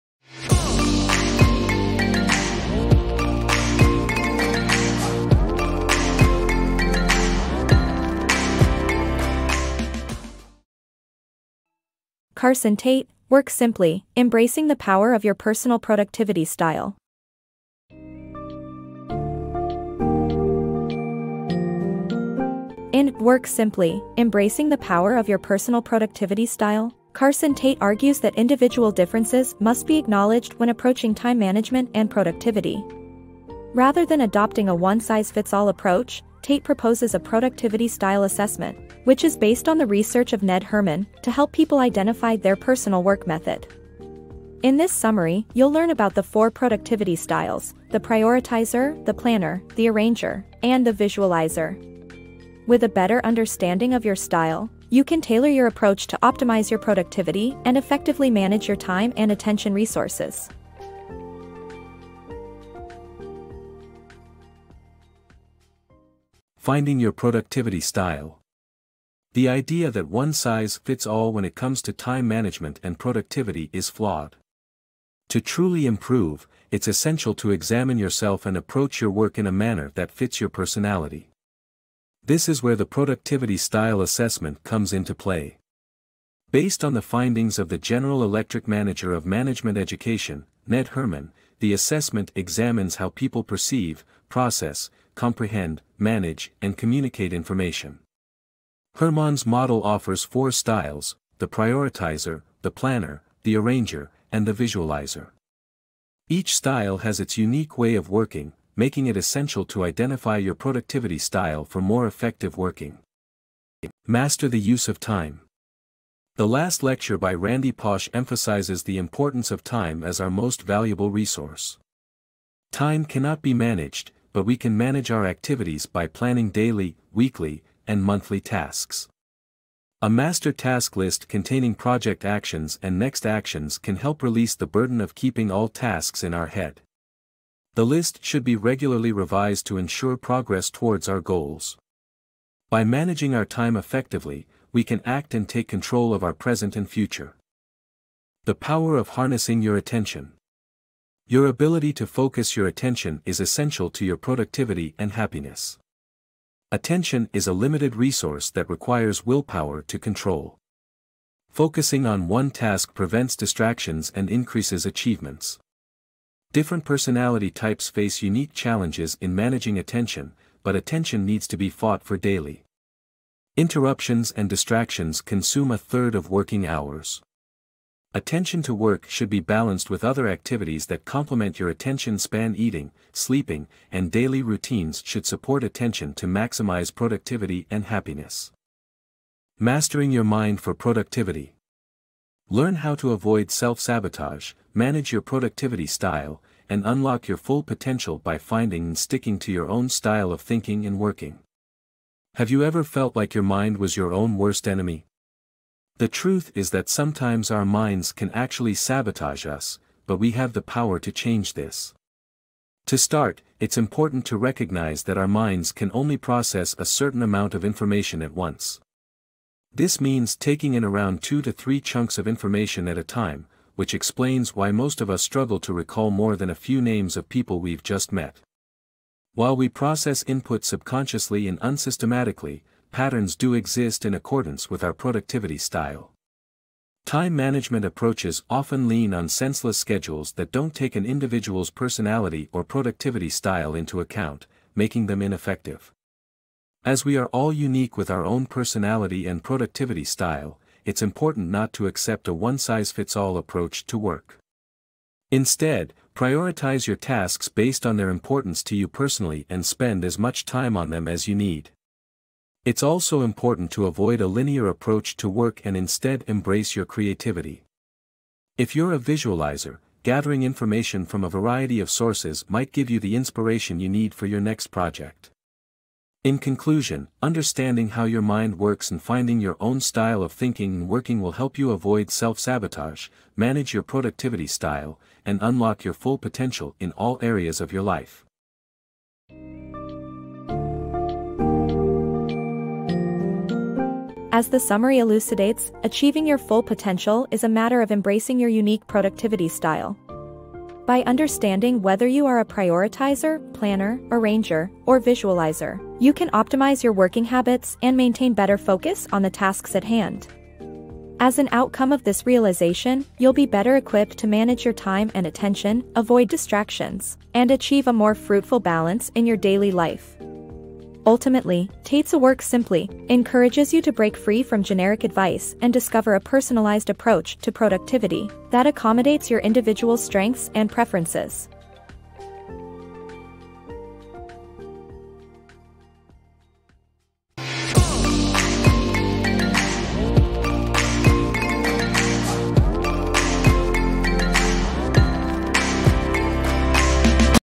Work Simply by Carson Tate: 5 Minute Summary
*BOOK SUMMARY*

TITLE - Work Simply: Embracing the Power of Your Personal Productivity Style

AUTHOR - Carson Tate

DESCRIPTION:
📖 Feeling like you're always busy but not achieving anything? Work Simply helps you identify what's holding you back and provides a step-by-step guide to improving your productivity, based on your unique strengths and preferences. Discover the tools, tactics, and techniques that work best for you and get more done in less time!

TIMESTAMPS:
⌚ 00:00 Introduction
⌚ 01:07 Finding Your Productivity Style
⌚ 01:54 Master the Use of Time
⌚ 02:29 The Power of Harnessing Your Attention
⌚ 03:10 Mastering Your Mind for Productivity
⌚ 04:49 Final Recap